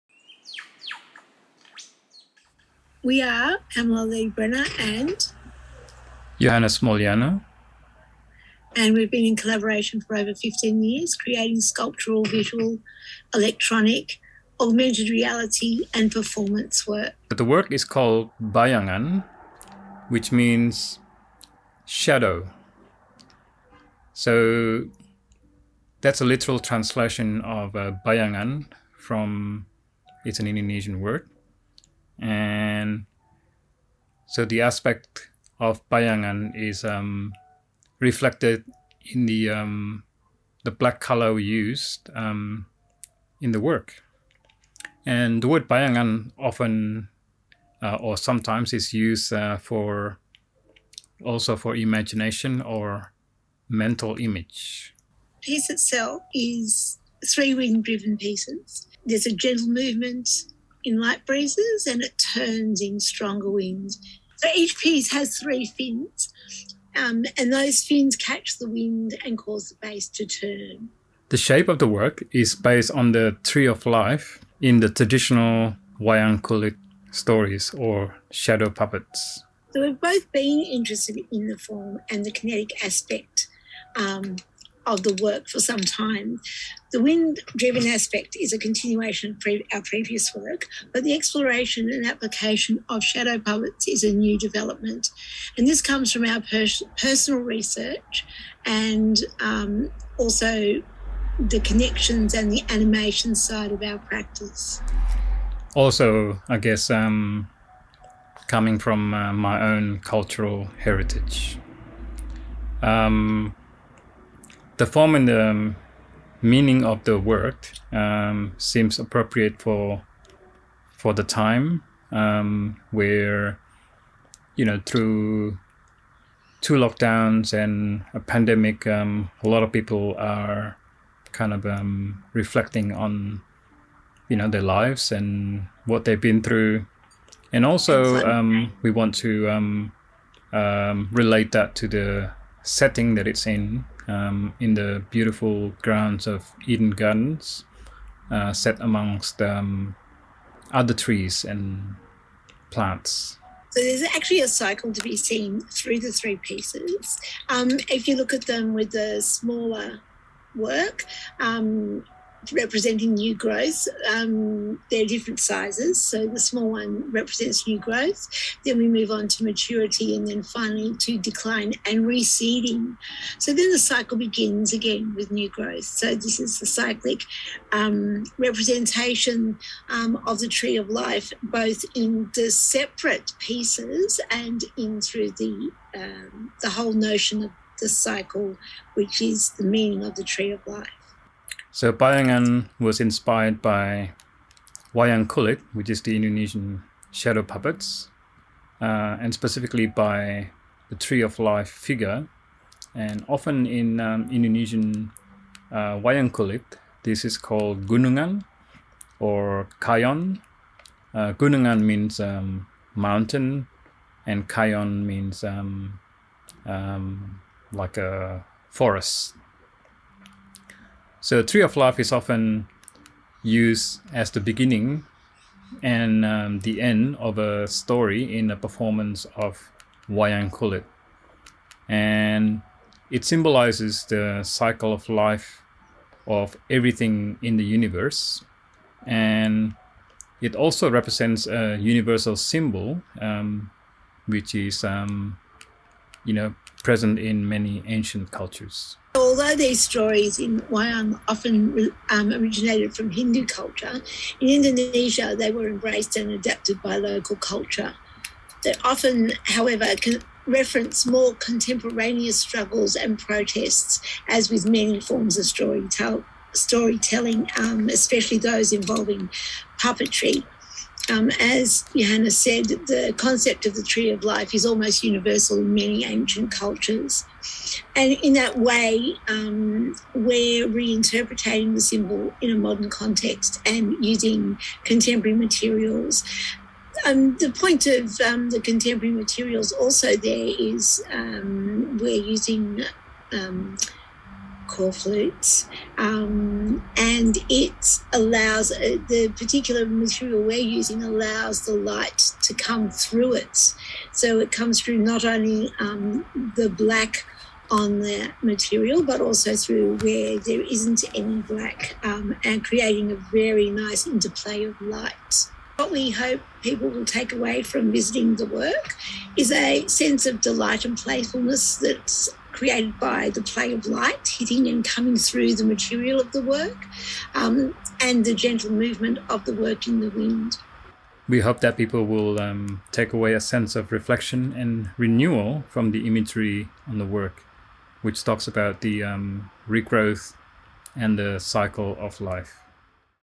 Eden Unearthed 2021 Bayangan- Pamela Lee Brenner and Johannes Muljana
23 . Bayangan, by Pamela Lee Brenner and Johannes Muljana, is a series of kinetic sculptures with shapes loosely referring to Javanese gunungan or kayon, a symbol used in the staging of wayang kulit (shadow puppetry), incorporating the tree of life symbol (pohon kahuripan). The word bayangan literally means shadow and, in the extended meaning, imagination. This sculpture is a play between the meanings of the symbols, imagining that we can take shelter and think beyond our daily activities, contemplating the larger question that is life.

Visit Eden Unearthed at Eden Gardens Macquarie Park from Nov 2021 to April 22.